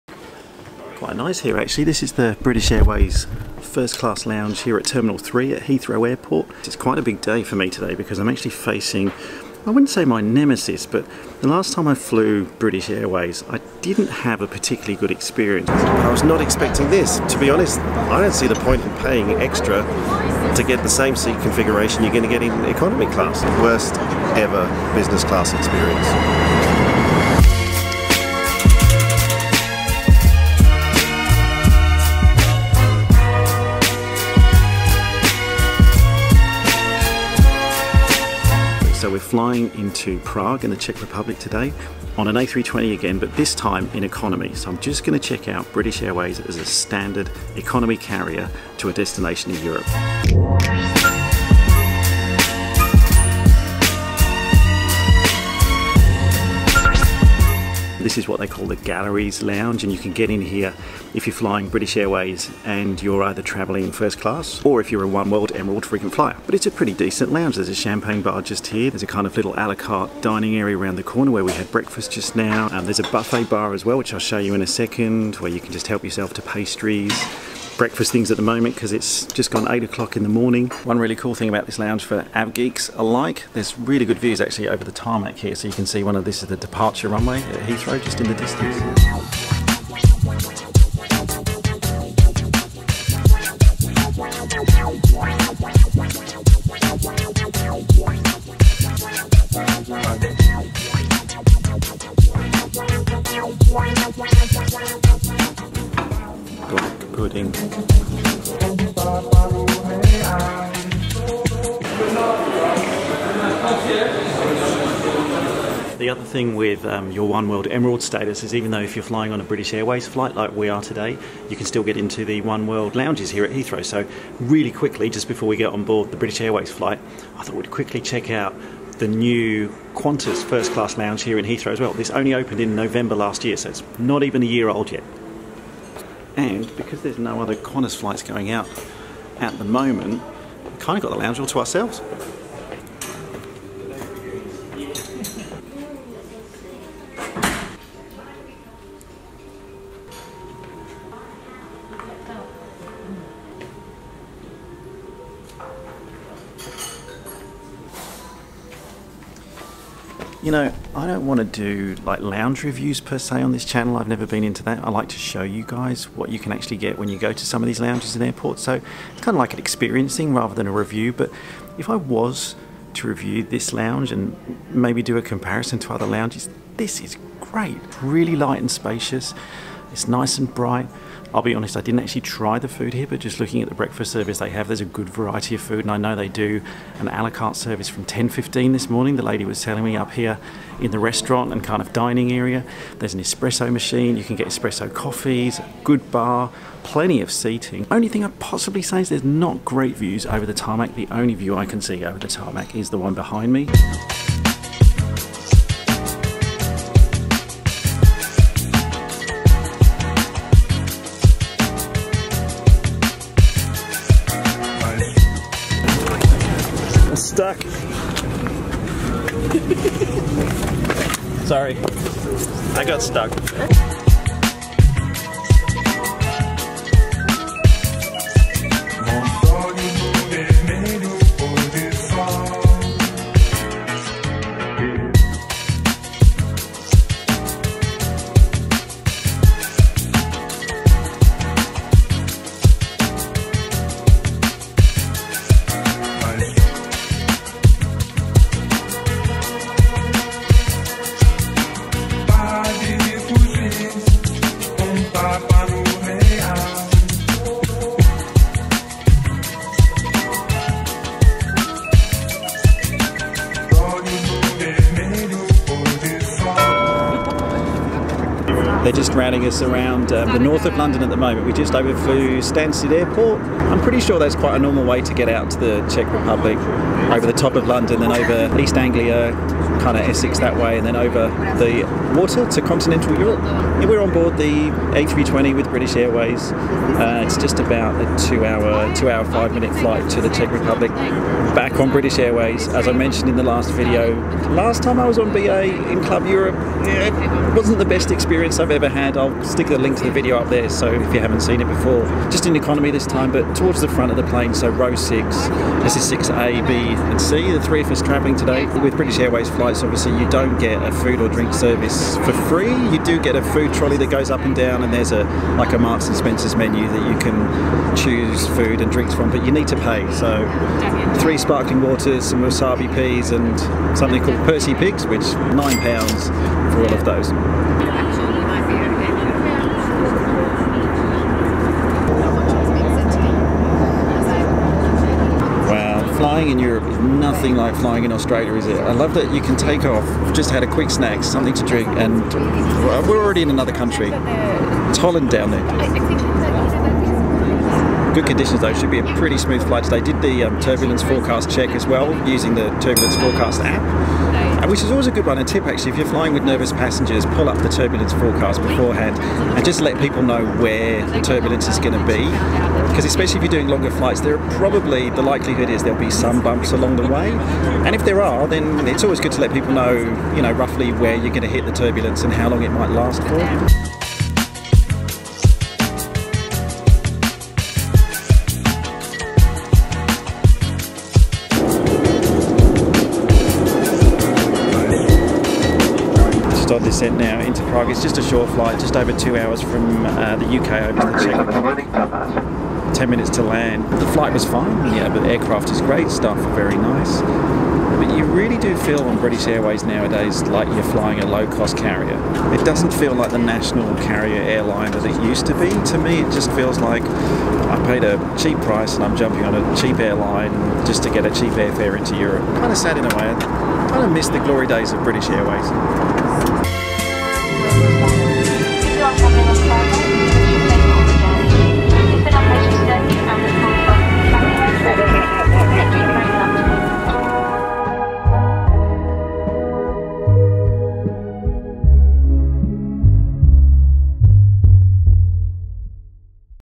British Airways First Class Lounge; London to Prague Euro Traveller
EP144: The Galleries - British Airways First Class Lounge at Heathrow Terminal 3, plus Euro Traveller London to Prague, and the new Qantas Lounge at Heathrow too. We're on an Airbus A320 to Prague, travelling with British Airways from Heathrow Airport.

Last time I flew British Airways wasn't my best experience, in Club Europe.